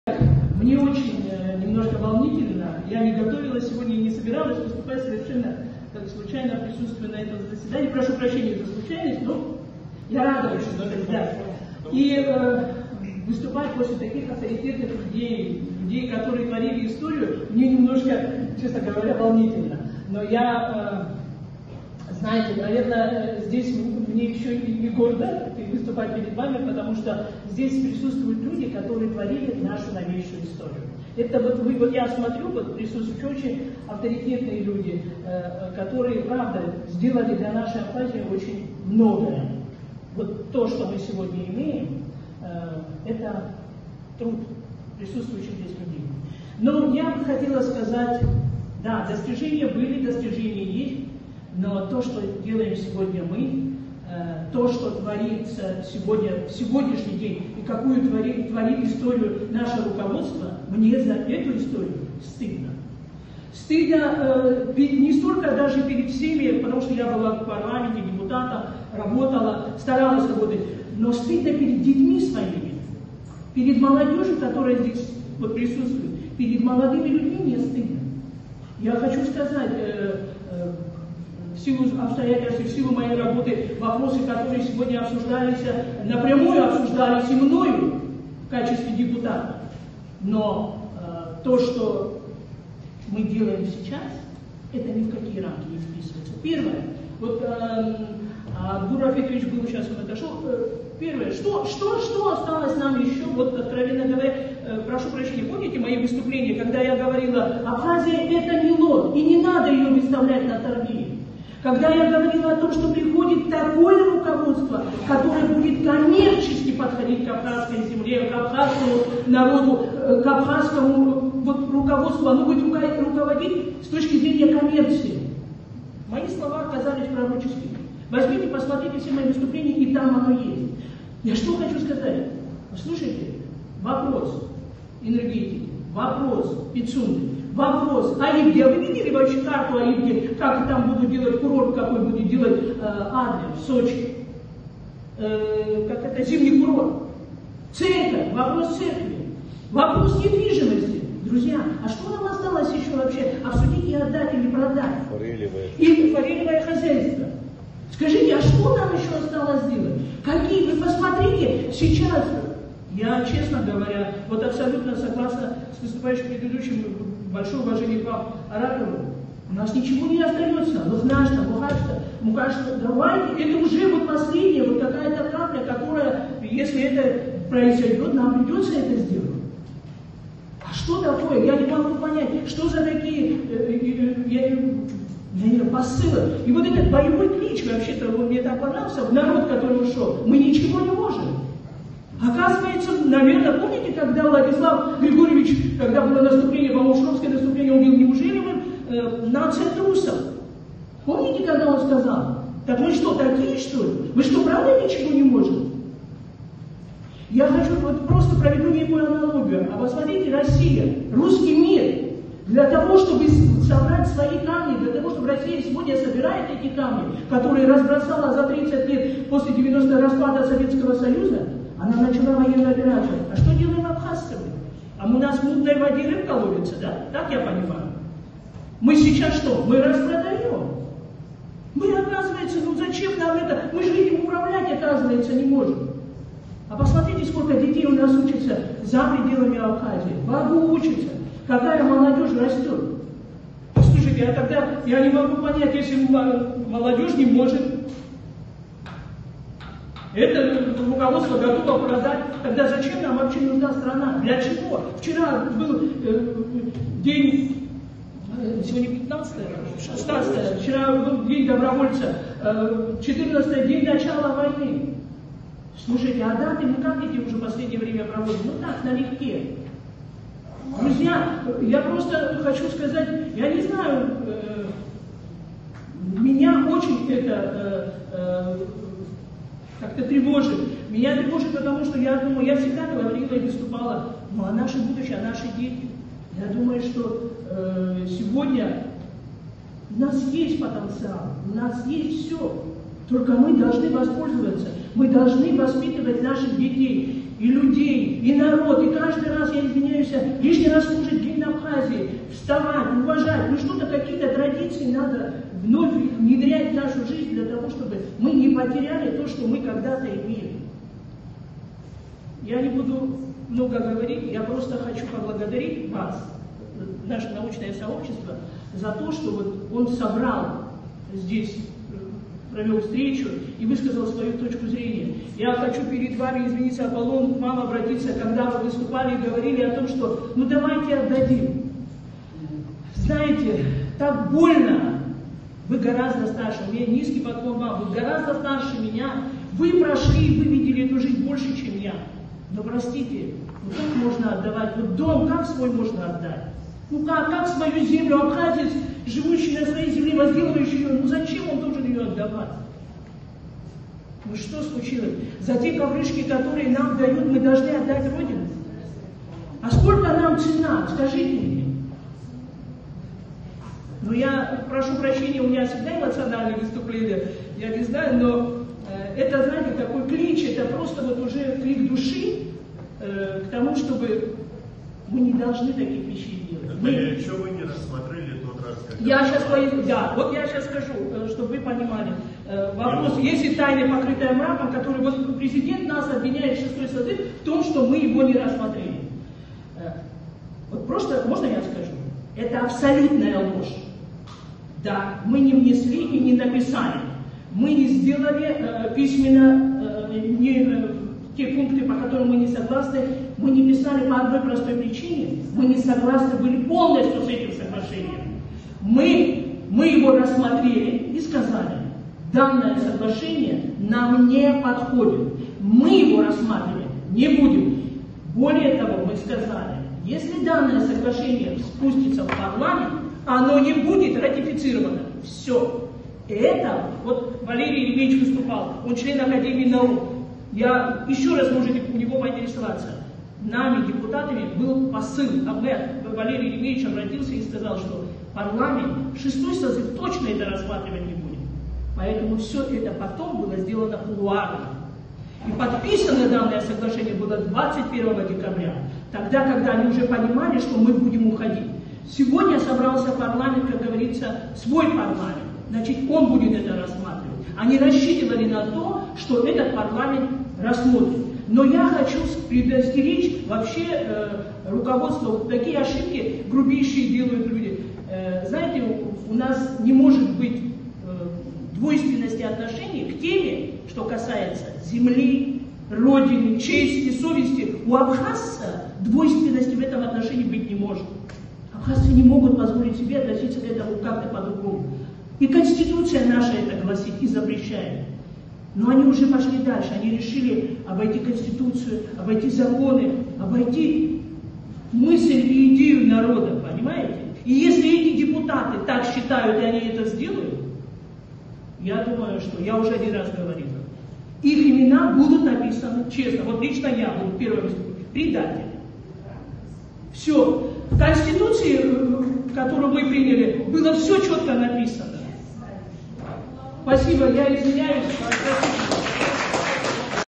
Натали Смыр : Президент нас обвиняет в том что, мы его не рассмотрели, это абсолютная ложь
Натали Смыр : Президент нас обвиняет в том что, мы его (соглашение по «Пицунде») не рассмотрели, это абсолютная ложь